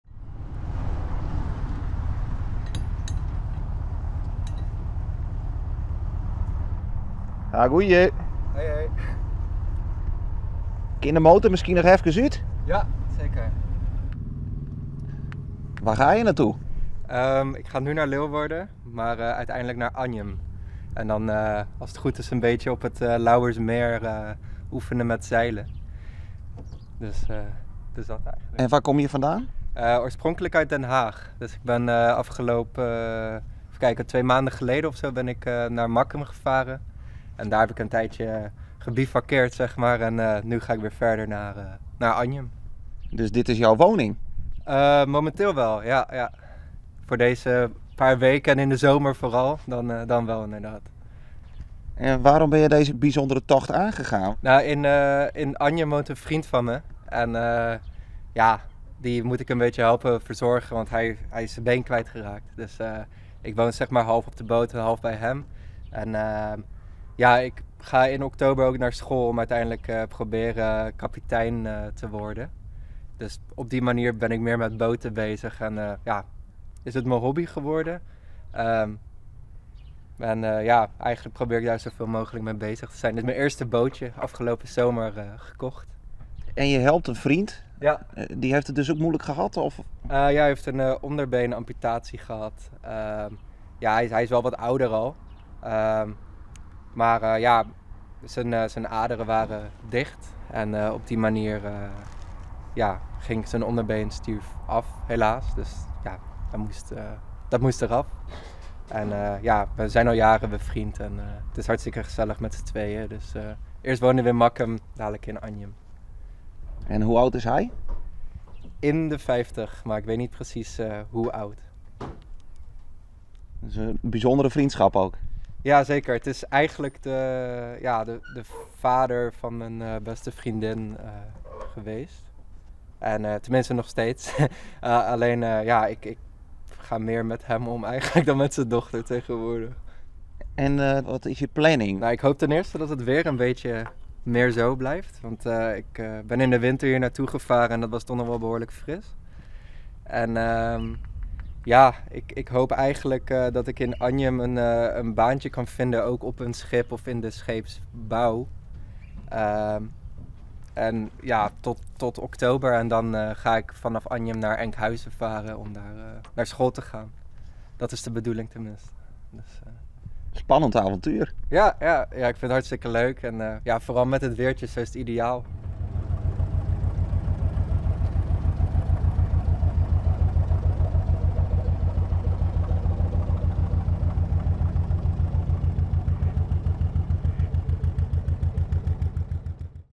HEA! Goeie Anjum
Nigel Koolhoven is met zijn boot onderweg na Anjum.